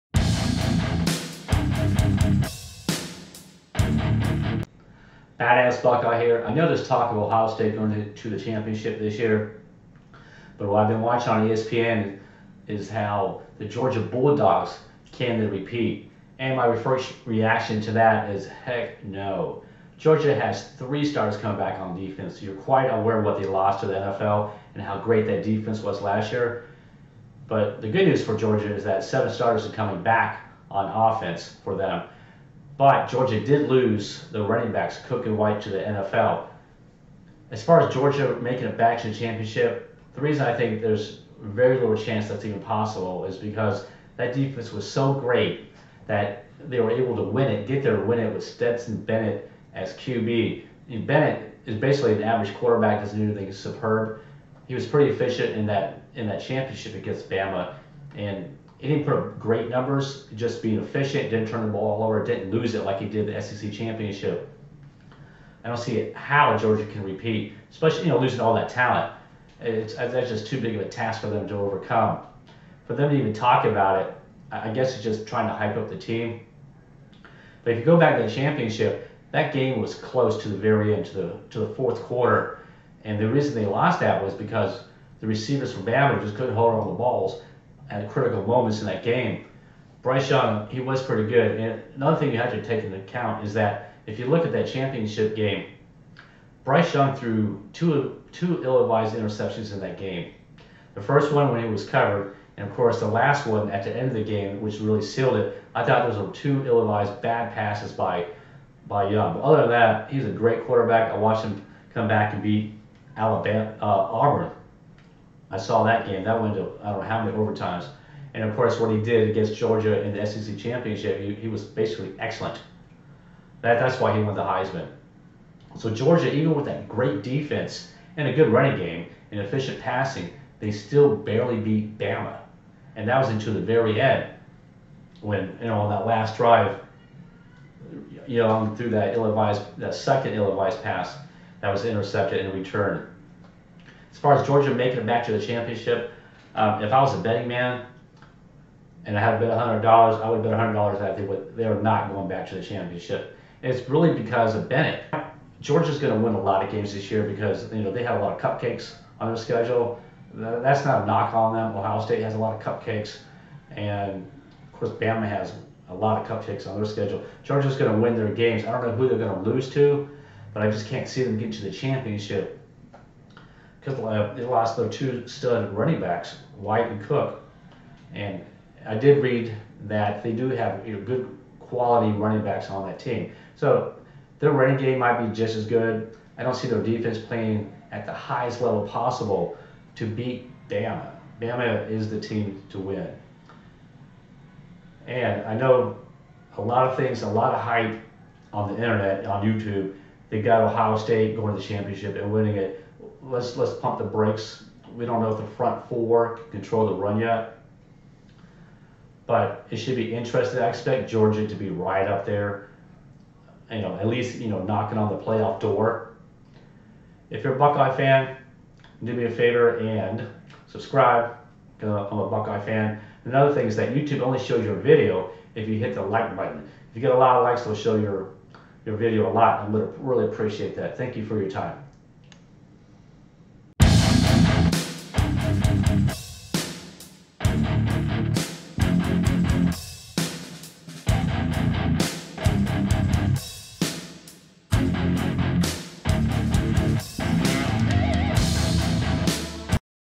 Can Georgia Bulldogs repeat as National Champions? - College Football
Georgia Bulldogs returning as Champions will be a tough task with QB Bennett because he is a game manager and not a game changer.  The defense will not be as potent as last year. Georgia needs to let the younger QBs play because it's the only way for them to get the experience for the future.